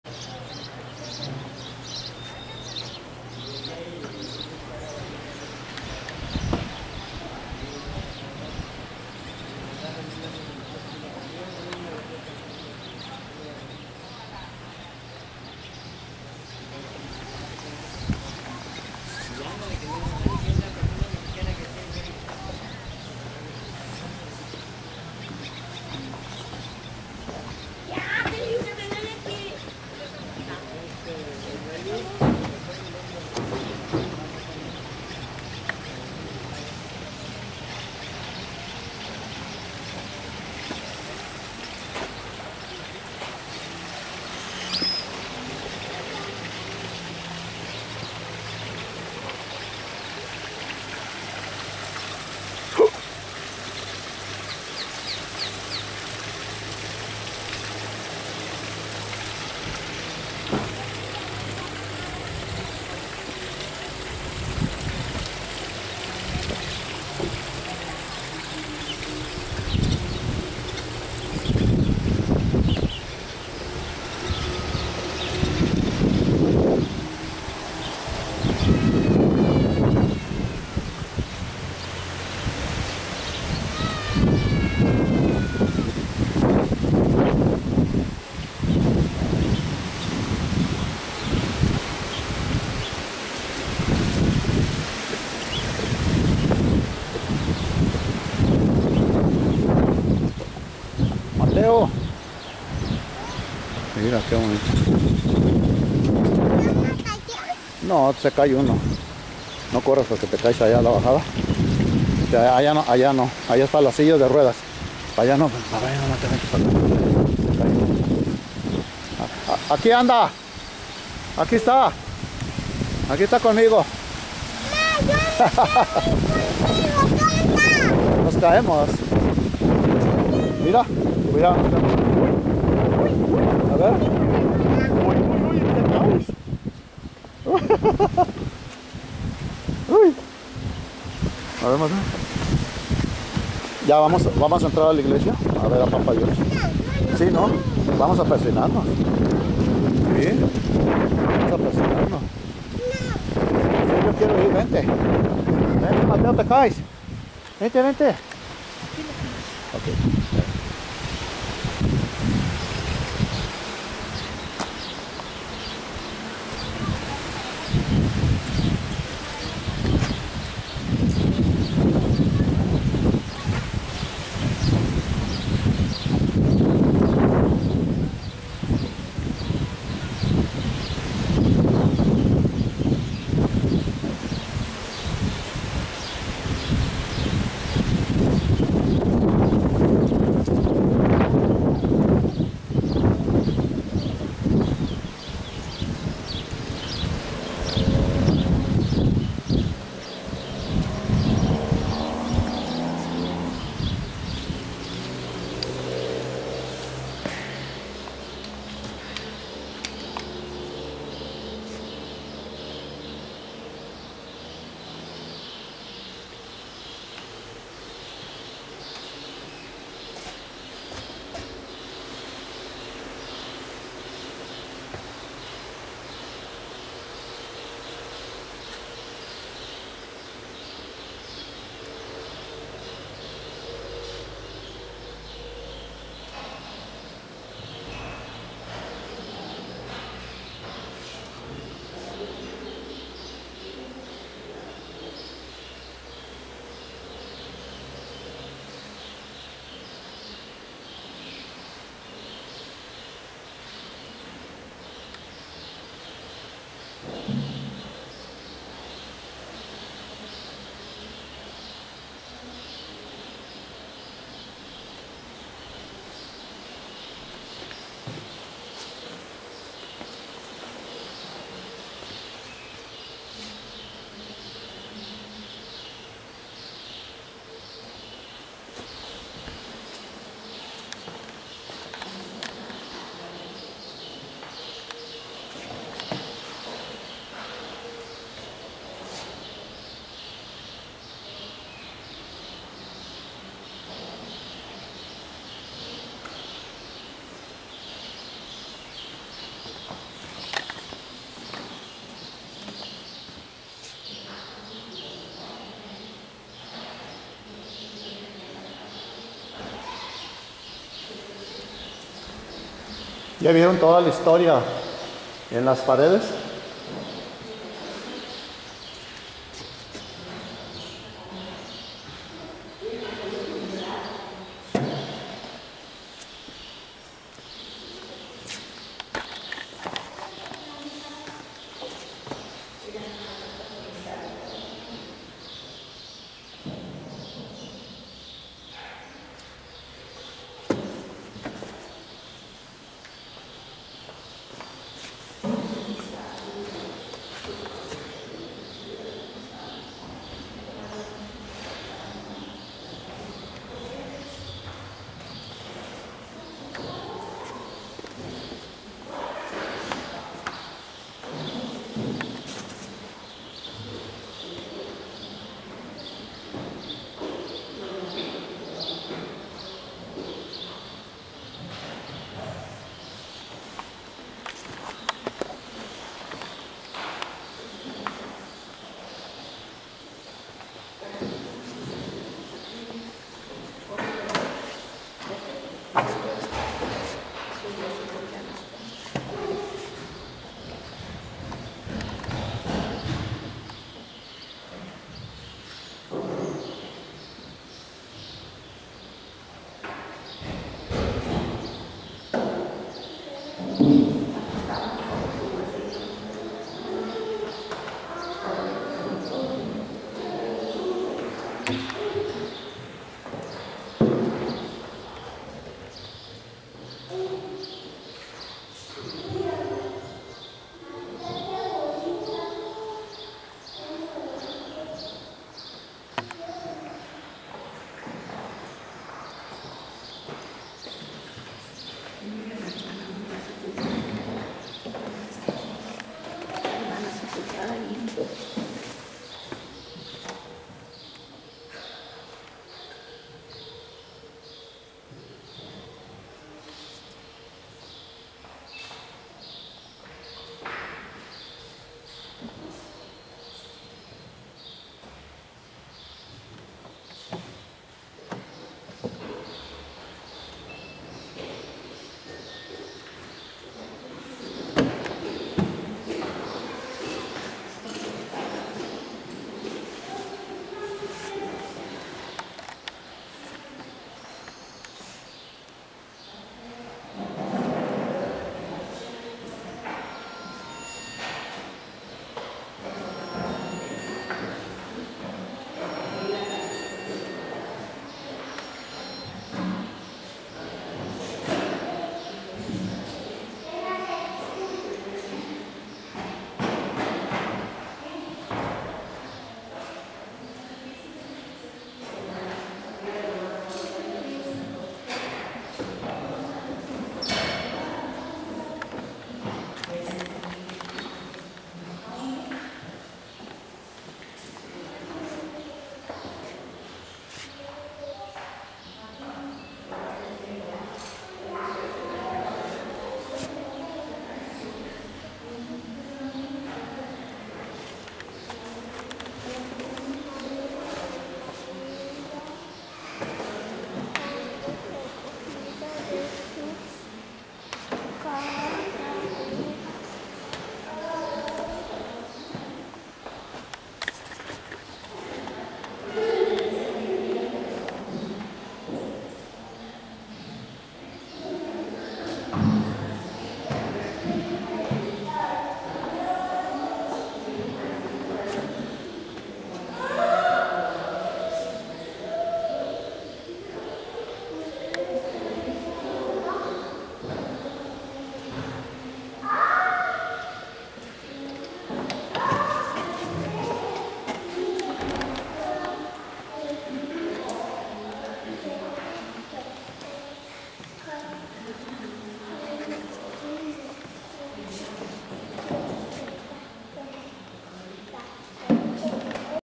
SANTO TORIBIO, IGLESIA LA MESITA
Visitando a Santo Toribio en su antigua iglesia de "La Mesita".  En su interior existe mucha información de la vida de este santo mártir de la Revolución Cristera y también de sus familiares.